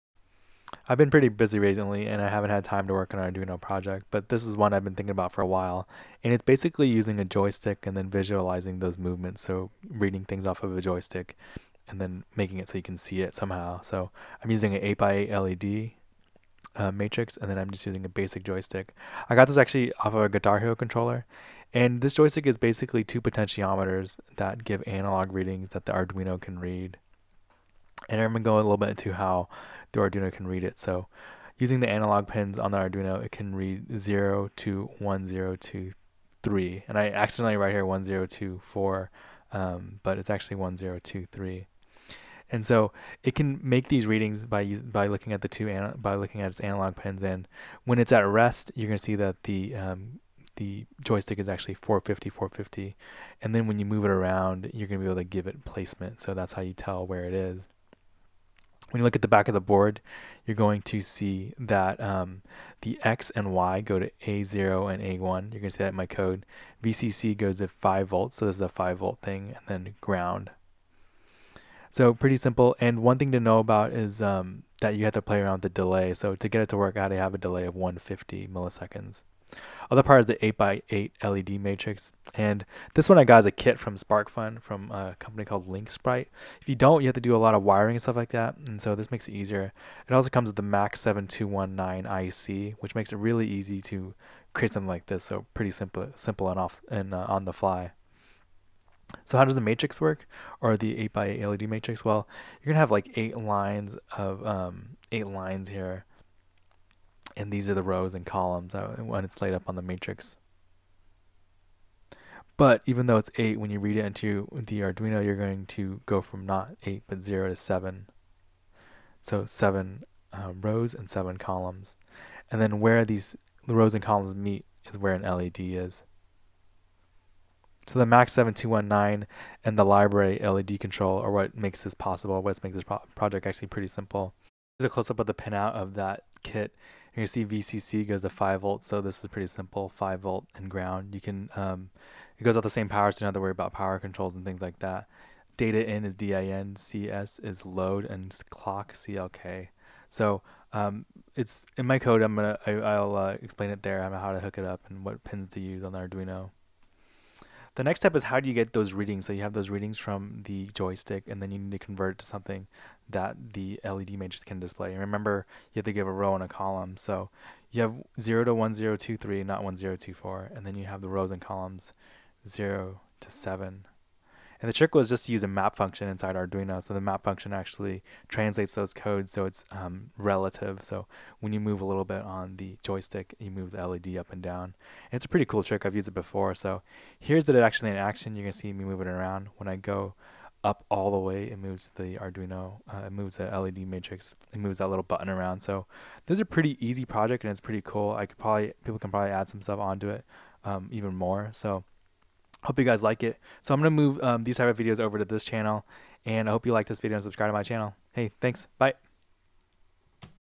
Arduino 2-axis Joystick and 8x8 LED tutorial
This is an old video but I'm just moving it. This is a simple example of how to attach an 8x8 LED to a 2 a-xis joystick and visualize the movements. It's simple and easy. the code